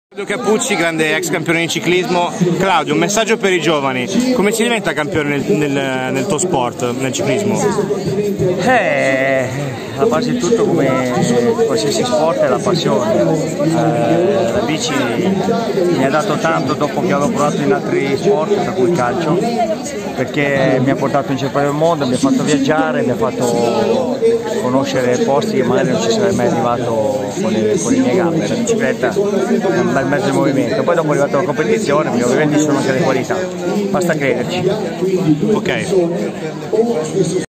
Claudio Chiappucci intervistato da SportSmall.it
L'ex campione di ciclismo nell'intervista ha parlato di come fare per diventare campioni